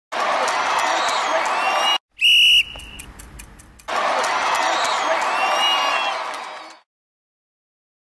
MILLIE THE PREDICTOR - Wales v Uruguay
Office pup Millie puts those psychic abilities of hers to use by predicting RWC2015 matches. If she predicts the correct outcome, we'll be giving away free books to those who guessed correctly along side her! To get involved, simply join in on our competitions each match day by heading to @BirlinnBooks or @ArenaSportBooks on Twitter. Good luck!